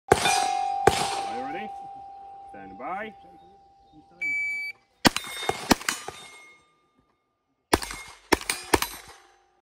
Steel Challenge, 13.06.2020, Outer Limits
Zawody strzeleckie Steel Challenge by Zapach Prochu, zgodnie z przepisami amerykańskiej federacji SCSA (Steel Challenge Shooting Association), podległej USPSA (United States Practical Shootind Association).